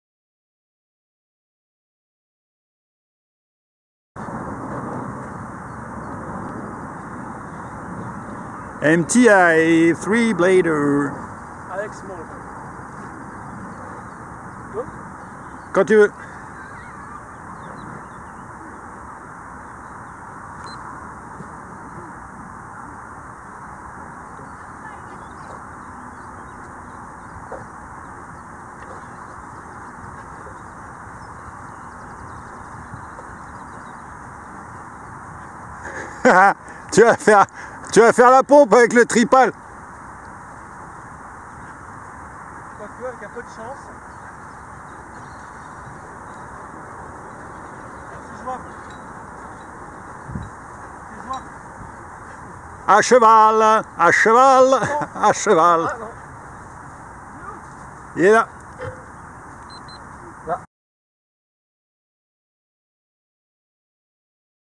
Throwing MTA Boomerang, 3 blader from Alex OPRI Mold .
This is about boomerang throwing , Maximum time Aloft with carbon boomerang. in this throw , a 3 blader made with Carbon and Alex OPRI Mold .